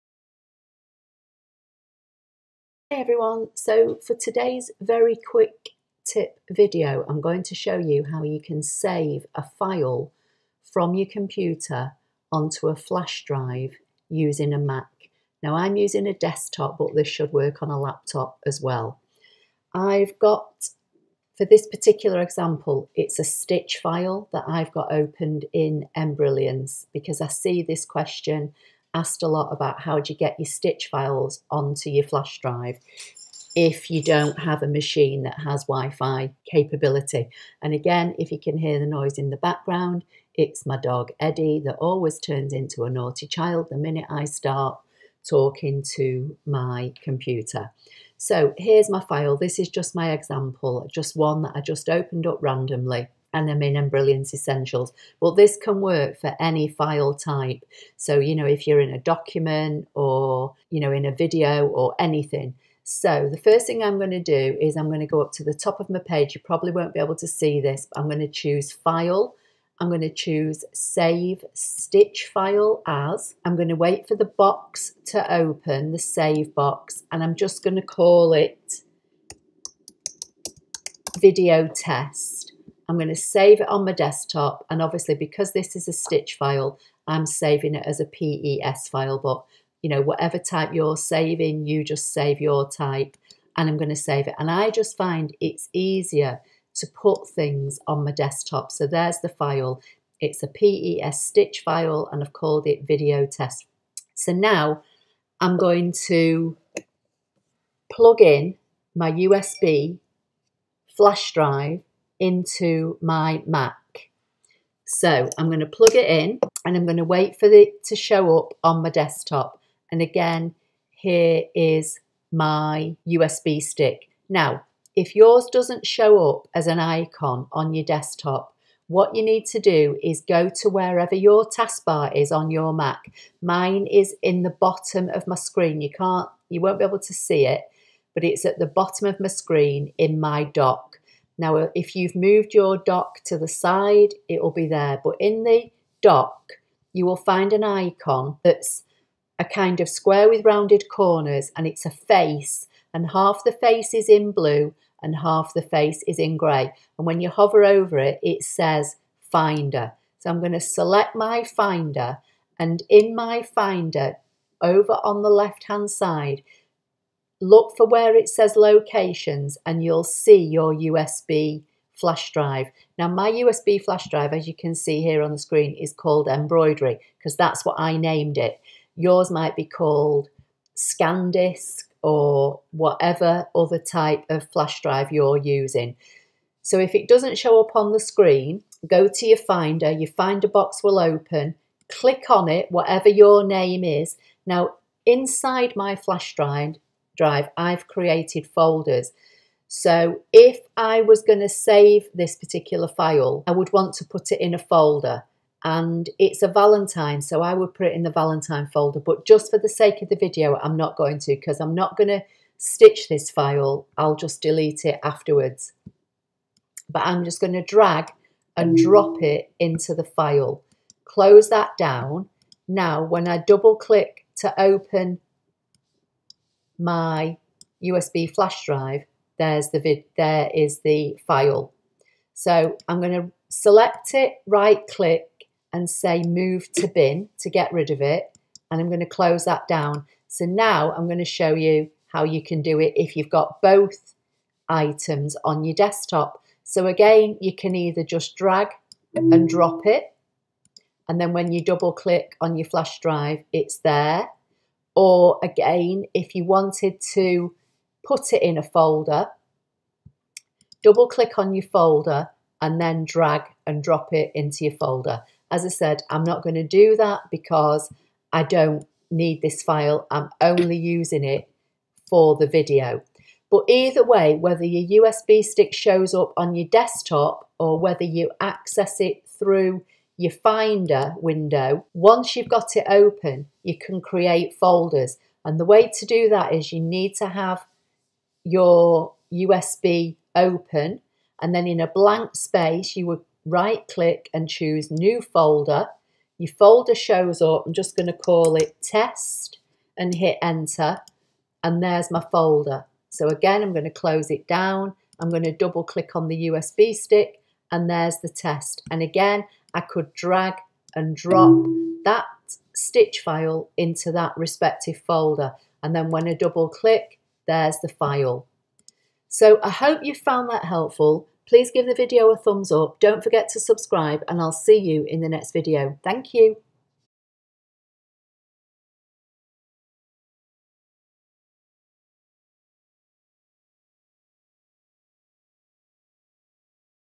How To Save An Embroidery Stitch File Onto A Flash drive/USB Stick On Mac
In todays applelover53 video i see this question asked a lot in various groups so I am showing you how easy it is to save a file onto a flash drive/usb stick on a Mac